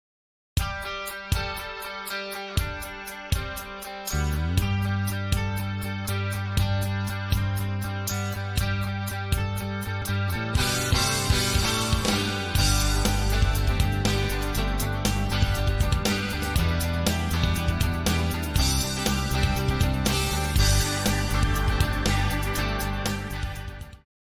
Now fund transfer is much Simpler - Start Using UPI for Buying & Selling Stocks
Alice Blue has introduced UPI Fund transfer so that investors and traders can make their transactions conveniently and more efficiently